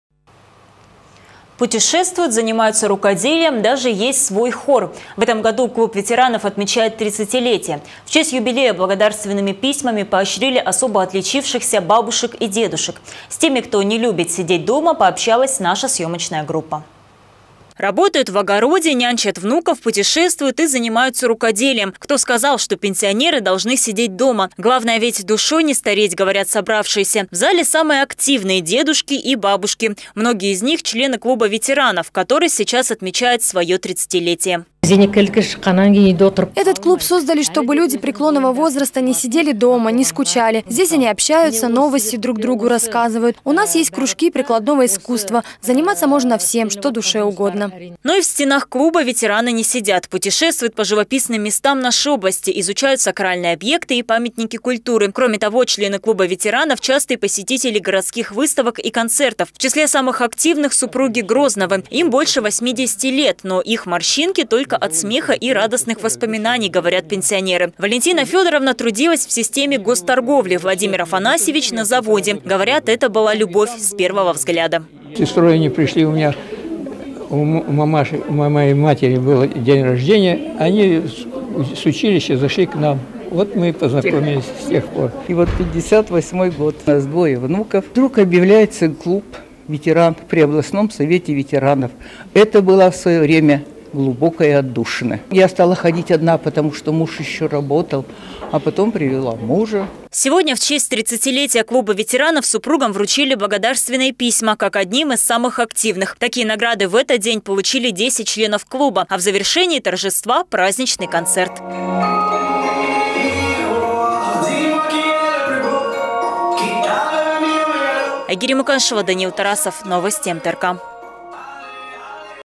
В этом году Клуб ветеранов отмечает своё 30-летие
Путешествуют, занимаются рукоделием, даже есть свой хор. В этом году клуб ветеранов отмечает 30-летие. В честь юбилея благодарственными письмами поощрили особо отличившихся бабушек и дедушек. С теми, кто не любит сидеть дома, пообщалась наша съёмочная группа.